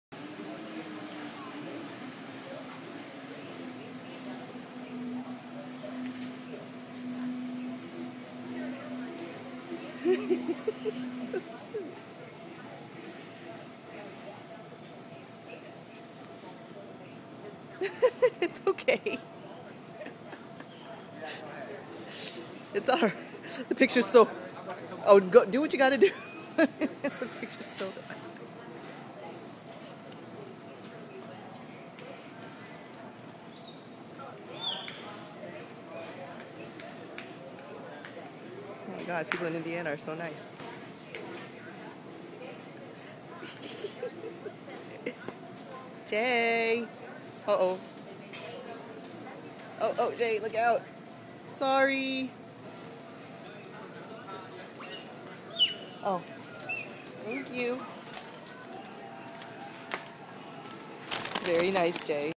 Running in Indianapolis
Jay gets to roam free on a light travel day at this airport layover.